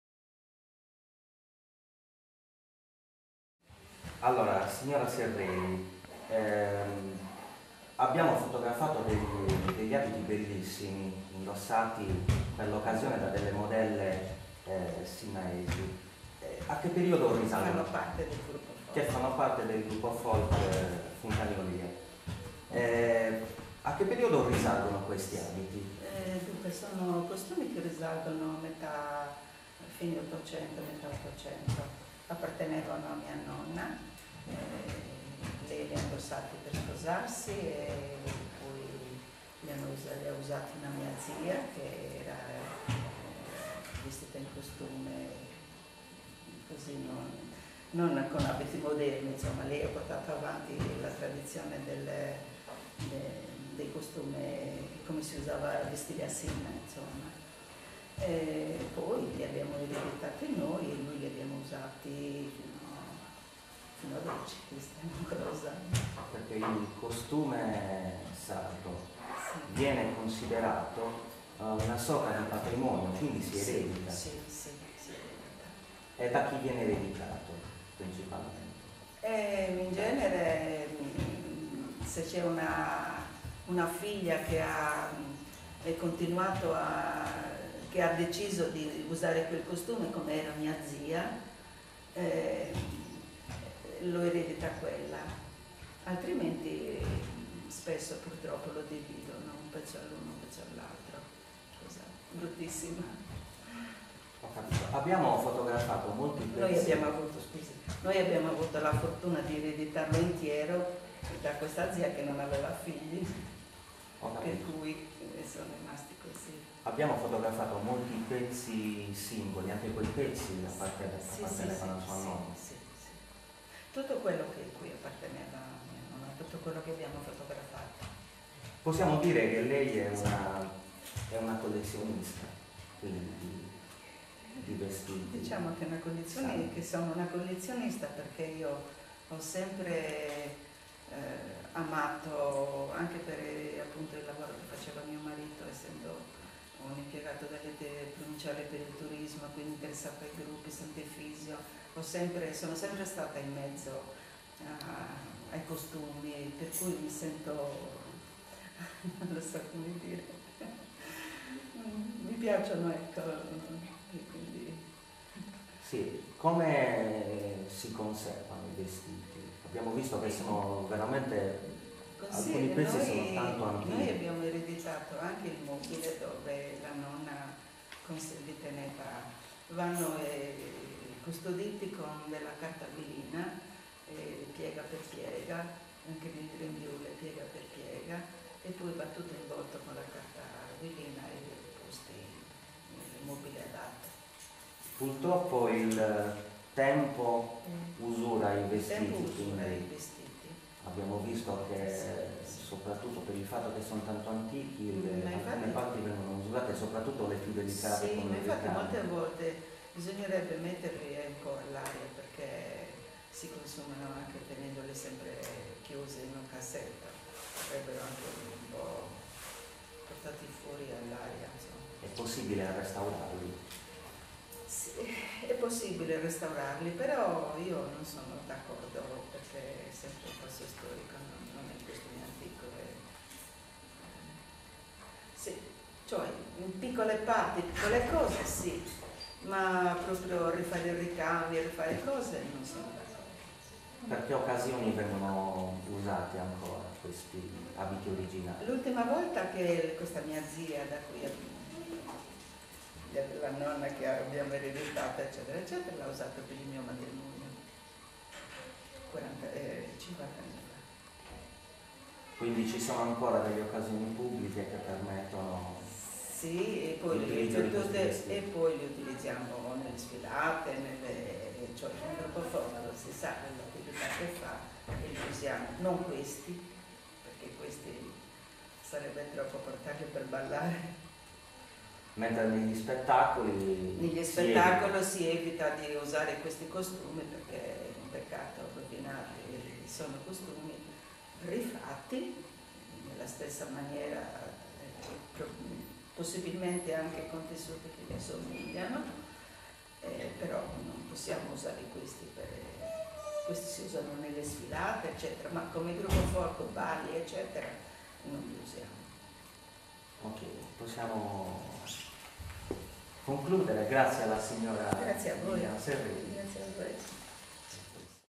Liliana Serreli, Sinnai - Il costume tradizionale
Liliana Serreli, appartenente all'associazione "Tradizioni popolari Funtana de Olia"
Abbiamo fotografato degli abiti bellissimi, indossati da modelle sinnaesi che fanno parte del gruppo folk "Funtana de Olia". A che periodo risalgono questi abiti?
Sono costumi che risalgono alla seconda metà dell'Ottocento; appartenevano a mia nonna che li ha indossati per sposarsi, poi sono passati ad una mia zia che ancora si vestiva in costume per preservare la tradizione. Noi li abbiamo ereditati e li usiamo ancora oggi.
Il costume sardo viene considerato una sorta di patrimonio, quindi si eredita?
Sì, normalmente si eredita.
Da chi viene ereditato?
Lo eredita un parente che ha deciso di usare quel costume, altrimenti, come capita spesso, si dividono i capi tra i parenti, una cosa bruttissima! Noi abbiamo avuto la possibilità di ereditarlo intero da questa nostra zia che non aveva figli.
Abbiamo fotografato molti capi singoli, anche quei pezzi appartenevano a sua nonna?
Tutto il materiale fotografato apparteneva a mia nonna.
Possiamo dire che lei è una collezionista di costumi sardi?
Diciamo che sono una collezionista. Io ho sempre amato i costumi, anche per via del lavoro che faceva mio marito, un impiegato provinciale per il turismo, quindi interessato ai gruppi che si occupavano di folklore (come ad esempio quelli che organizzavano la sfilata di Sant'Efisio).
Come si conservano i pezzi antichi del costume?
Noi abbiamo ereditato anche il mobile in cui mia nonna teneva gli abiti. Vanno custoditi con la carta velina che si inserisce tra un vestito e l'altro e tra le pieghe, poi si avvolge completamente nella carta velina e si ripone nei cassetti.
Purtroppo il tempo usura i vestiti e le parti più delicate, come i ricami, si rovinano. É possibile restaurarli?
Innanzitutto bisogna dire che un vestito, di tanto in tanto, deve essere portato all'aria aperta. Io non sono d'accordo a restaurare un vestito perché si commetterebbe un falso storico. Si può intervenire in qualche piccola parte, ma ricostruire completamente orli e ricami non è possibile.
In quali occasioni vengono ancora usati questi pezzi di abiti originali?
L'ultima volta li usò mia zia, per il mio matrimonio, cinquant'anni fa.
Quindi ci sono ancora occasioni pubbliche in cui questi vestiti vengono utilizzati?
Qualche volta vengono usati nelle sfilate, ma per ballare utilizziamo soprattutto delle riproduzioni in quanto gli abiti originali sono troppo preziosi e delicati.
Quindi negli spettacoli si usano delle riproduzioni?
Sì certo, sarebbe un peccato rovinarli. I vestiti vengono riprodotti in modo identico con tessuti similari.